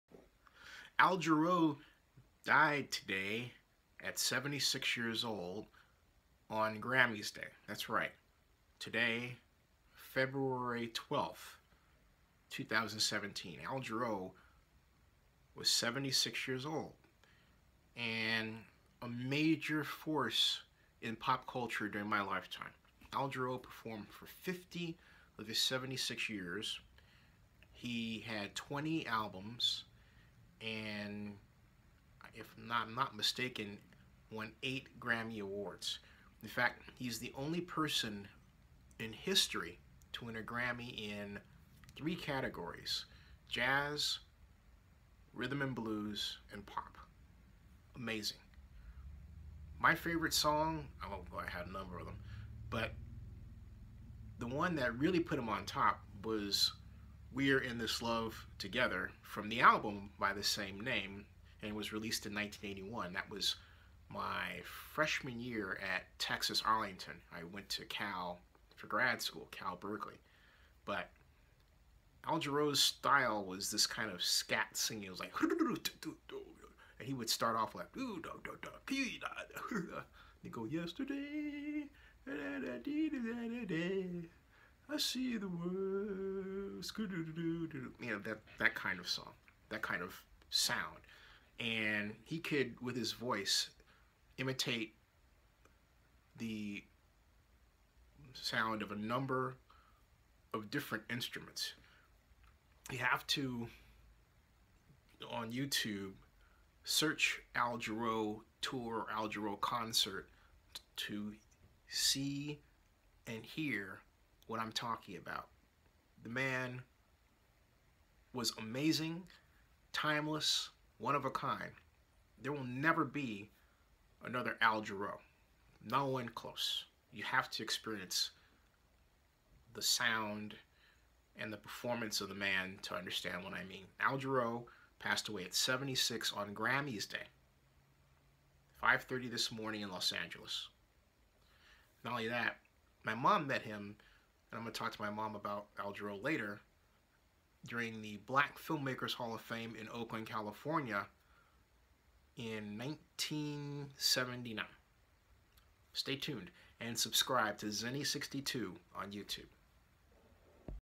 Al Jarreau Passed Away On Grammys Day RIP
Zennie62 Al Jarreau Passed Away On Grammys Day RIP GRAMMYs. The legend was 76 years old. Mr Jarreau remains the only singer to win a Grammy in three categories: Jazz, R&B, Pop Culture.